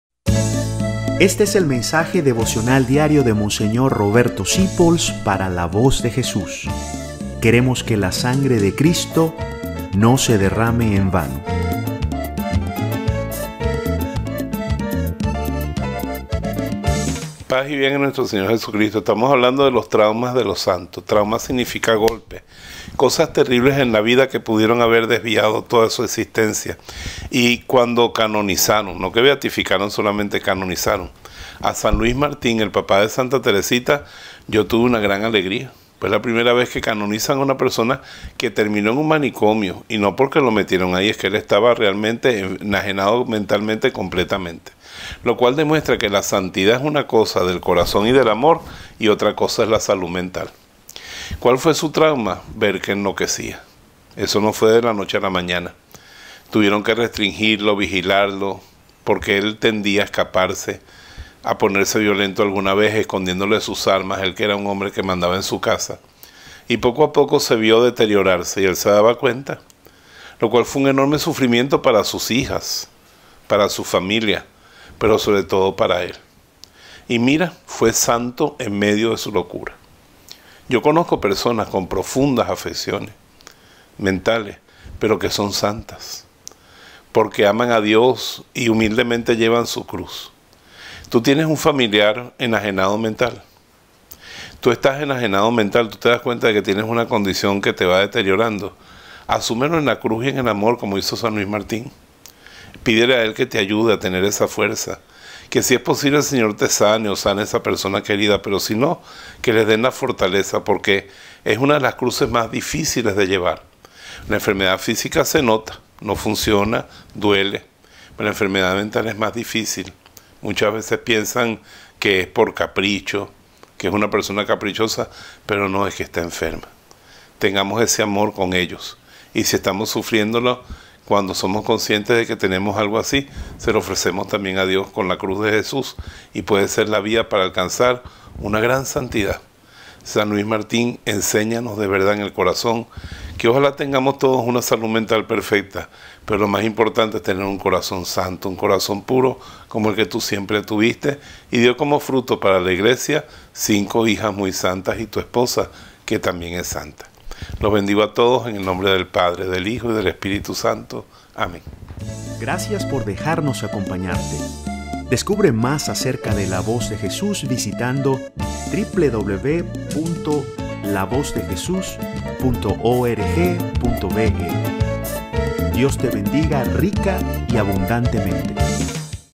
San Luis Martín (demencia) - Los Traumas de los Santos : 25 de Abril 2023 #230425
Mensaje devocional diario por Monseñor Roberto Sipols para La Voz de Jesús

40 traumas, 40 santos